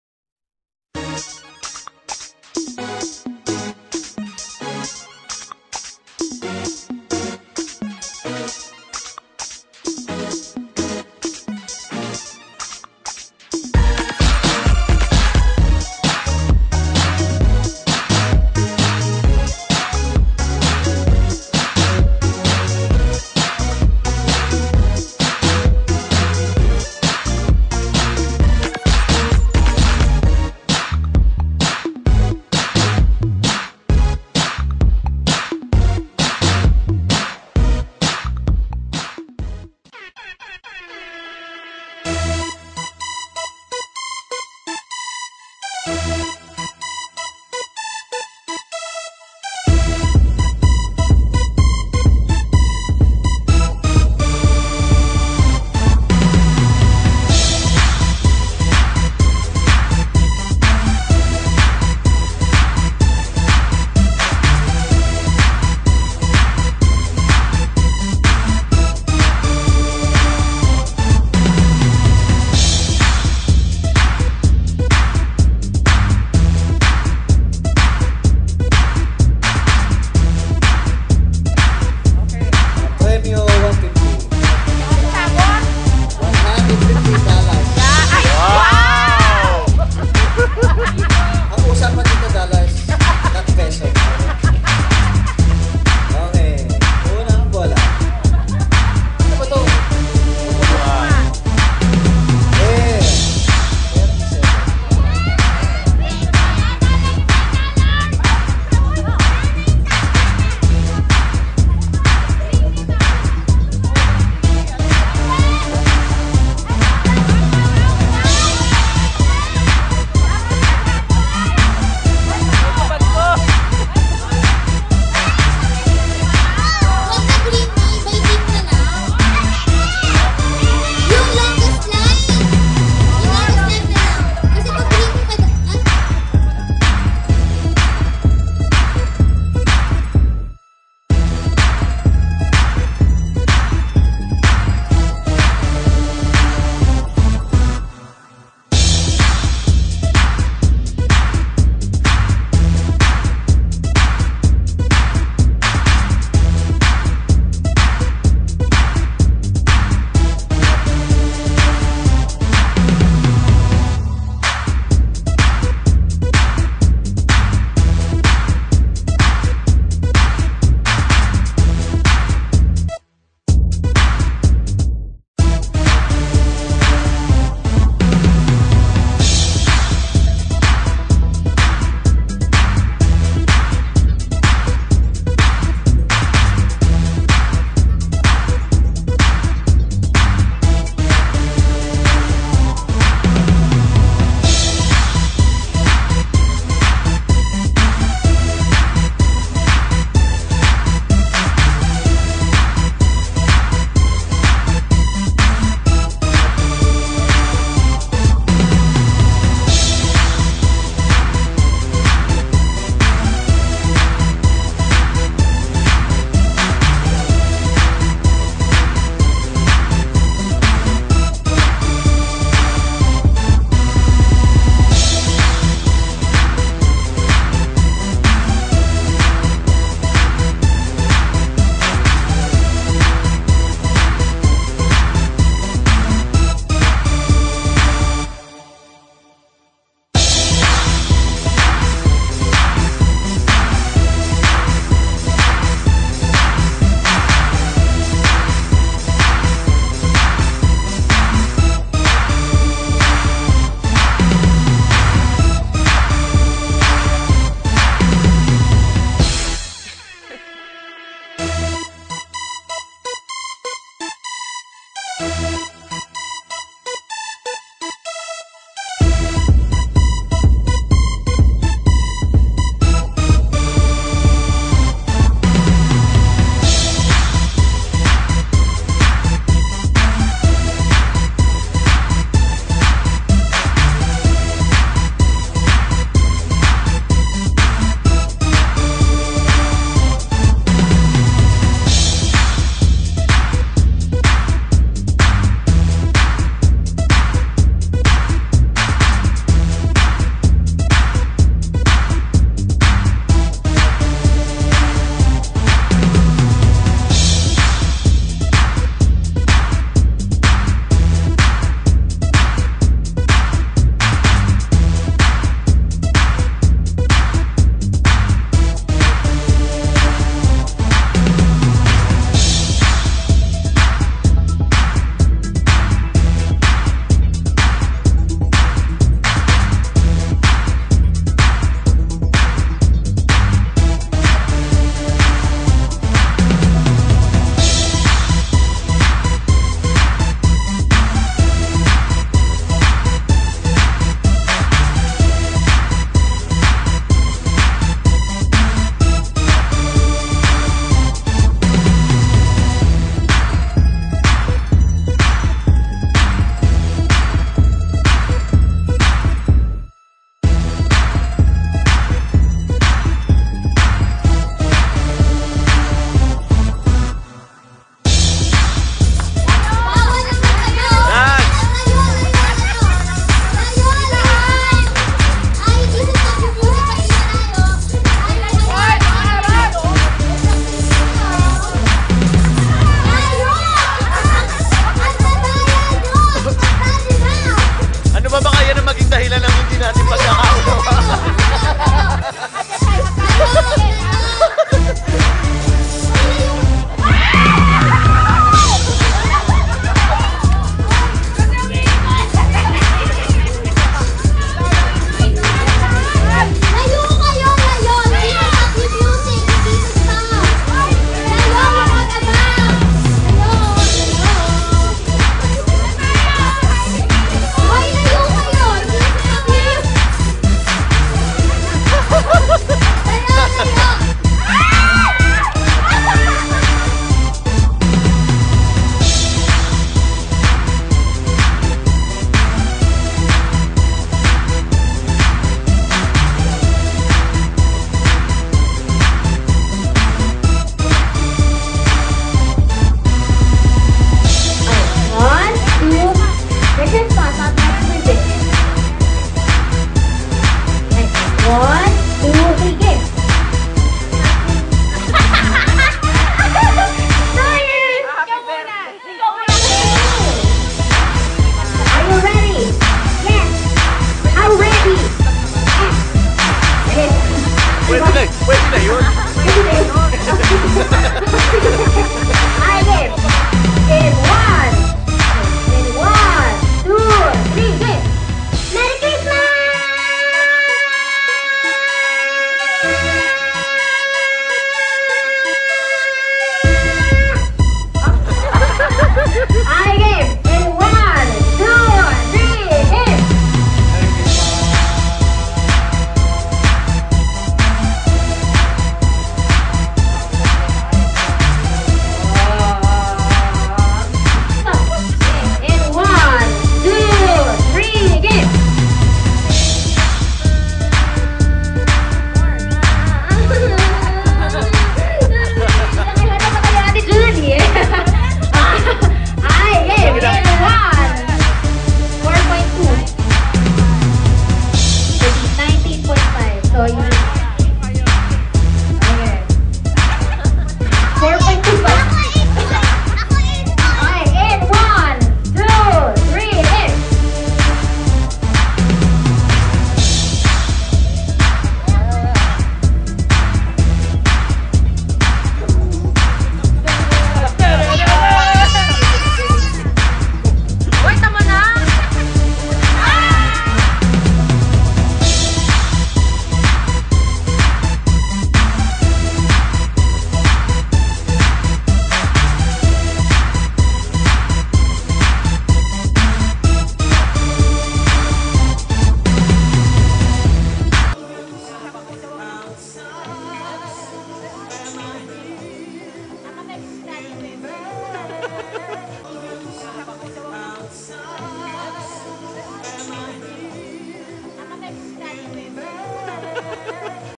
Family Reunion 2009
Family reunion on my mother's side.
December 24, 2009
Christmas Eve
Camella Homes, Alabang
Muntinlupa city